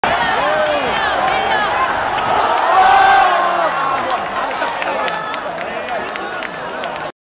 南華亞協之旅對蒙通聯之片段7
里奧有機會的一射 不過我影唔清楚